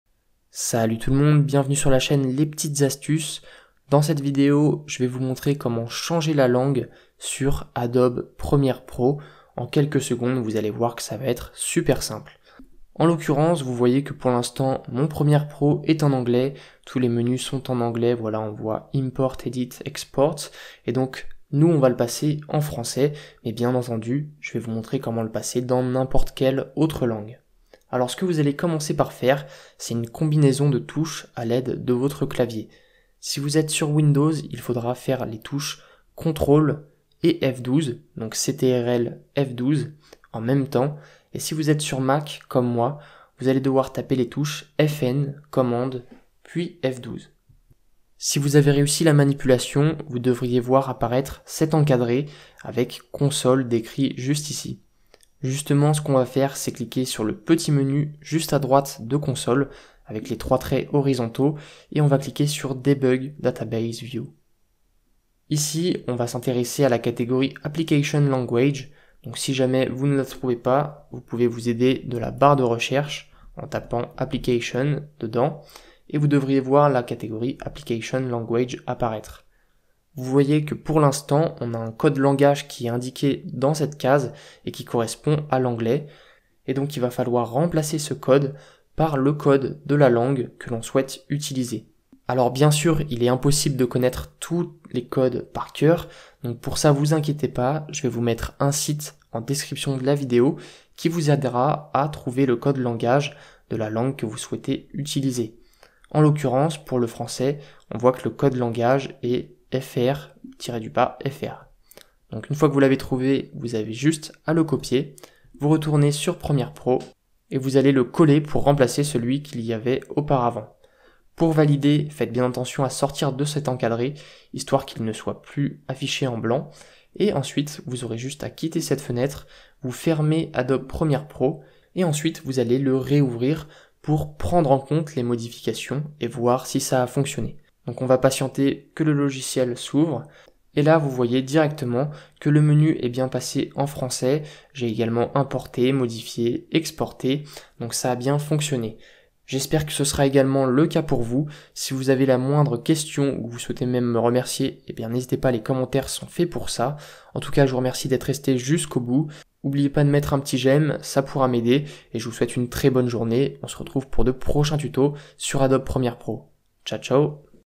Comment changer de langue sur Adobe Premiere Pro
Dans cette vidéo, je vous montre comment changer la langue sur le logiciel Adobe Premiere Pro. Celle-ci est souvent en anglais par défault. Je vous montre comment avoir Adobe Premiere Pro en français. Bien sûr, vous pourrez mettre n'importe quelle autre langue.